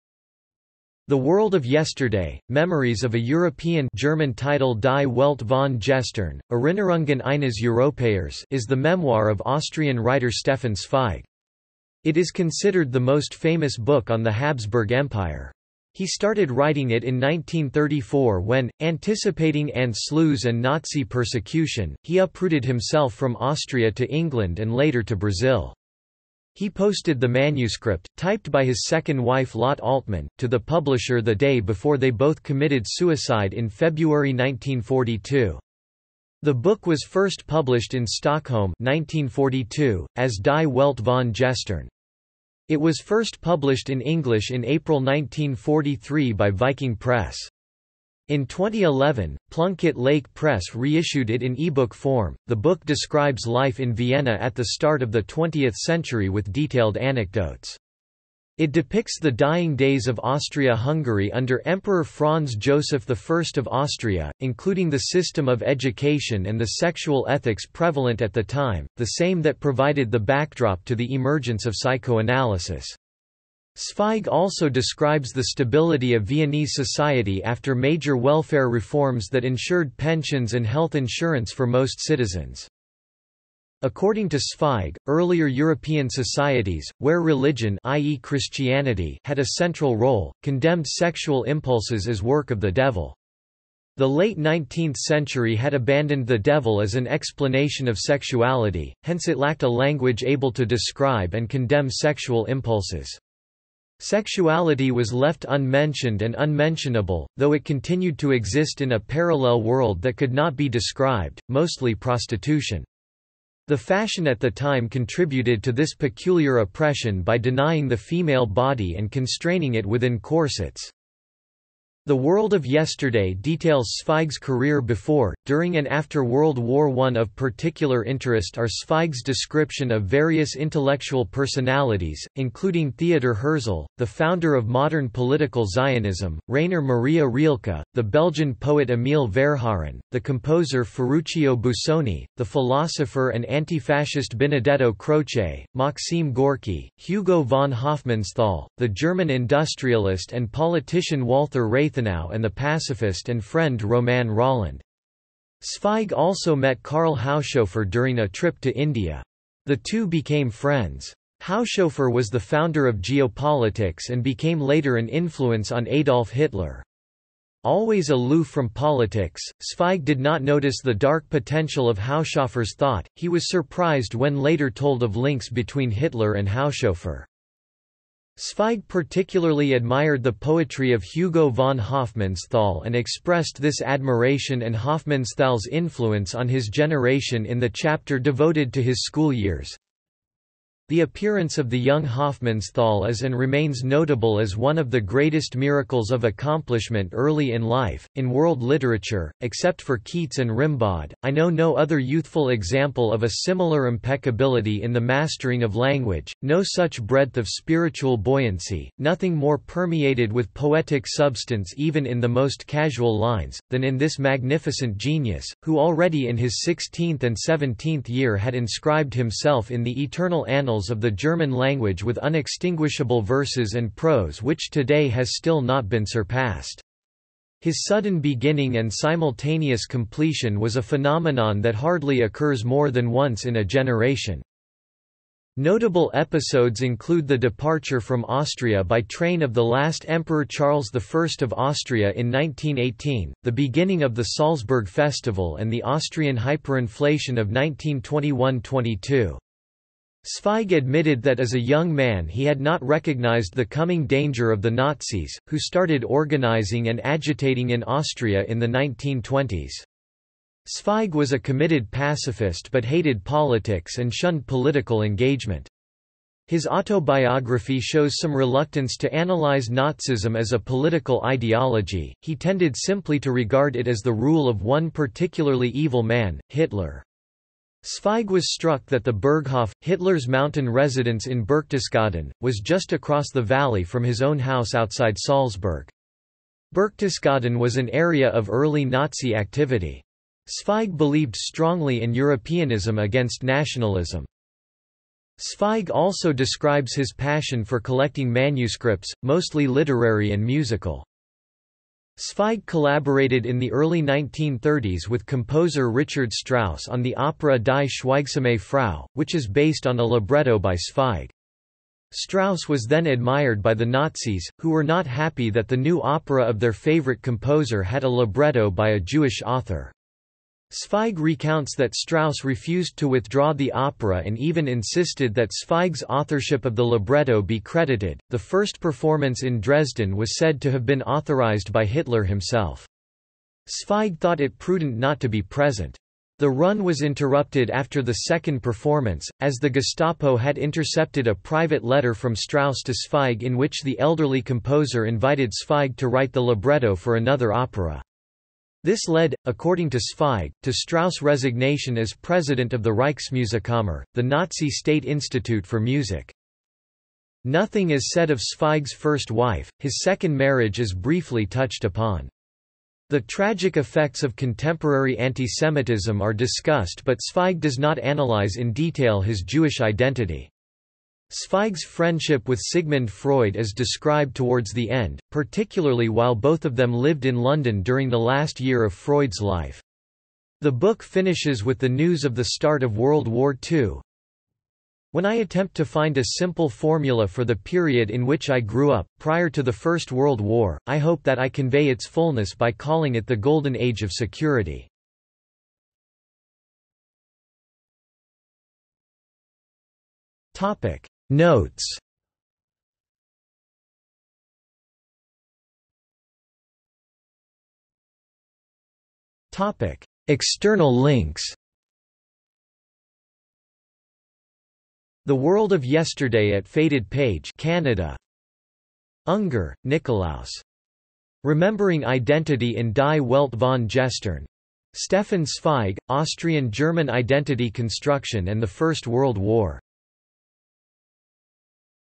The World of Yesterday | Wikipedia audio article
00:06:36 Notes
00:06:45 External links

- Socrates

SUMMARY
The World of Yesterday: Memories of a European (German title Die Welt von Gestern: Erinnerungen eines Europäers) is the memoir of Austrian writer Stefan Zweig. It is considered the most famous book on the Habsburg Empire. He started writing it in 1934 when, anticipating Anschluss and Nazi persecution, he uprooted himself from Austria to England and later to Brazil.  He posted the manuscript, typed by his second wife Lotte Altmann, to the publisher the day before they both committed suicide in February 1942. The book was first published in Stockholm (1942), as Die Welt von Gestern. It was first published in English in April 1943 by Viking Press. In 2011, Plunkett Lake Press reissued it in eBook form.The book describes life in Vienna at the start of the 20th century with detailed anecdotes. It depicts the dying days of Austria-Hungary under Emperor Franz Joseph I of Austria, including the system of education and the sexual ethics prevalent at the time, the same that provided the backdrop to the emergence of psychoanalysis. Zweig also describes the stability of Viennese society after major welfare reforms that ensured pensions and health insurance for most citizens.
According to Zweig, earlier European societies, where religion (i.e. Christianity) had a central role, condemned sexual impulses as work of the devil. The late 19th century had abandoned the devil as an explanation of sexuality; hence it lacked a language able to describe and condemn sexual impulses. Sexuality was left unmentioned and unmentionable, though it continued to exist in a parallel world that could not be described, mostly prostitution. The fashion at the time contributed to this peculiar oppression by denying the female body and constraining it within corsets.

The World of Yesterday details Zweig's career before, during and after World War I. Of particular interest are Zweig's description of various intellectual personalities, including Theodor Herzl, the founder of modern political Zionism, Rainer Maria Rilke, the Belgian poet Emile Verhaeren, the composer Ferruccio Busoni, the philosopher and antifascist Benedetto Croce, Maxim Gorky, Hugo von Hofmannsthal, the German industrialist and politician Walther Rathenau and the pacifist and friend Romain Rolland. Zweig also met Karl Haushofer during a trip to India. The two became friends. Haushofer was the founder of geopolitics and became later an influence on Adolf Hitler. Always aloof from politics, Zweig did not notice the dark potential of Haushofer's thought; he was surprised when later told of links between Hitler and Haushofer.
Zweig particularly admired the poetry of Hugo von Hofmannsthal and expressed this admiration and Hofmannsthal's influence on his generation in the chapter devoted to his school years:

The appearance of the young Hofmannsthal is and remains notable as one of the greatest miracles of accomplishment early in life; in world literature, except for Keats and Rimbaud, I know no other youthful example of a similar impeccability in the mastering of language, no such breadth of spiritual buoyancy, nothing more permeated with poetic substance even in the most casual lines, than in this magnificent genius, who already in his sixteenth and seventeenth year had inscribed himself in the eternal annals of the German language with unextinguishable verses and prose which today has still not been surpassed. His sudden beginning and simultaneous completion was a phenomenon that hardly occurs more than once in a generation.
Notable episodes include the departure from Austria by train of the last Emperor Charles I of Austria in 1918, the beginning of the Salzburg festival and the Austrian hyperinfla ...